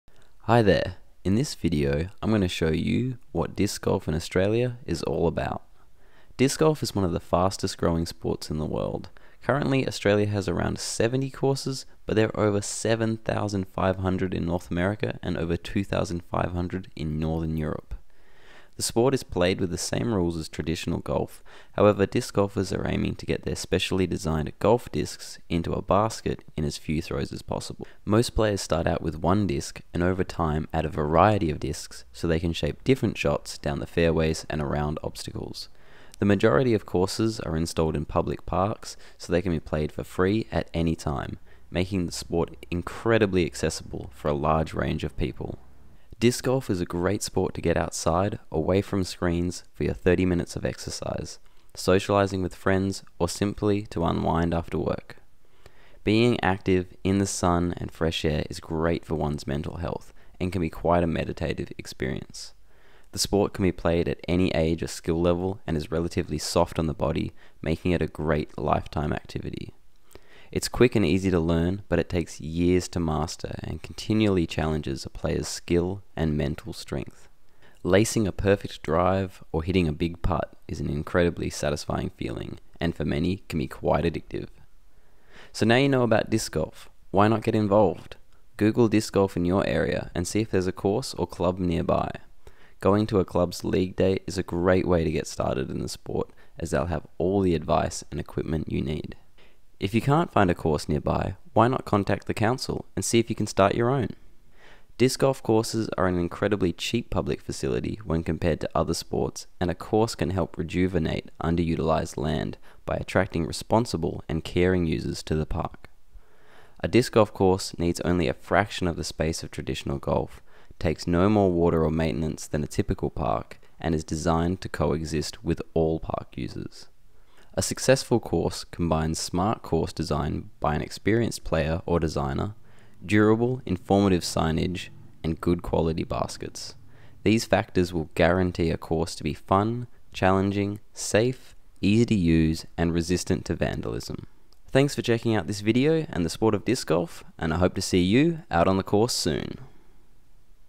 Disc Golf in Australia
An introductory video to Disc Golf and how to get involved! This video features courses from all over the country including Melbourne, Geelong, Hobart, Perth and regional Queensland.

If you would like more info on Why Disc Golf?